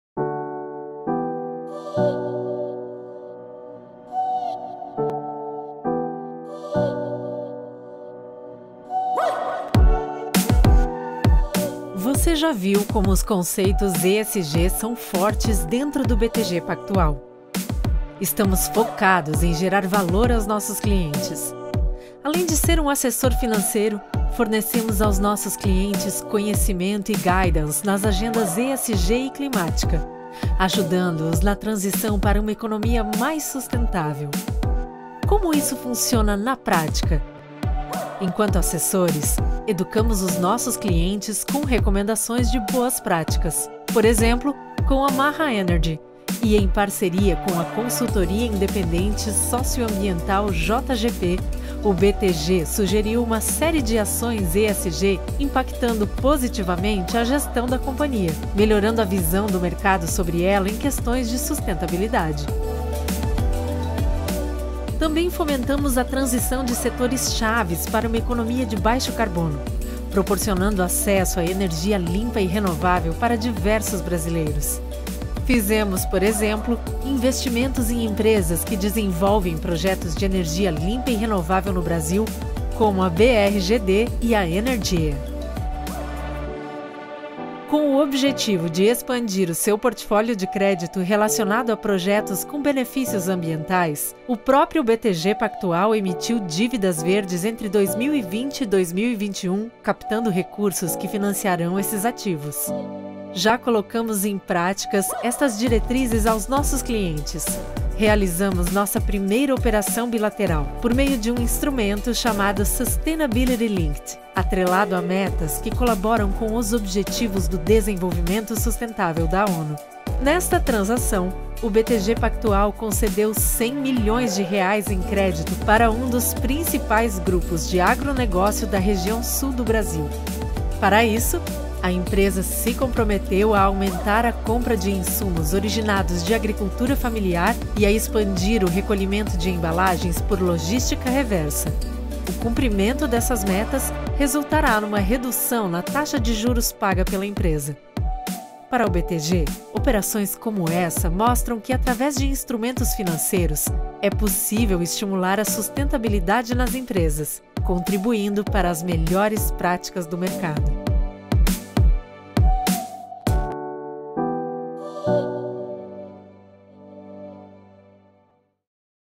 ESG no BTG Pactual: consultorias para as empresas melhorarem sua performance em sustentabilidade
Os conceitos ESG são fortes dentro do BTG Pactual. Na prática, o banco fornece conhecimento e guidance nas agendas ESG e climática, ajudando na transição para uma economia mais sustentável. Assista ao vídeo para conhecer exemplos práticos de consultorias fornecidas pelo BTG que geraram ótimos resultados de mercado.